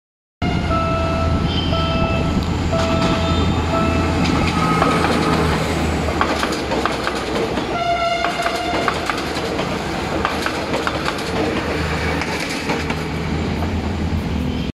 PNR INKA DMU 8104 passing Malugay crossing
My first time to do some train spottings after a seven month hiatus due to the pandemic.  While waiting for my mom at the Makati Central Fire Station, i decided to go to the nearby Malugay and Buendia crossings.

Configuration:
MeC-7, TC-7, TC-8, MeC-8.

1:12 pm, October 27, 2020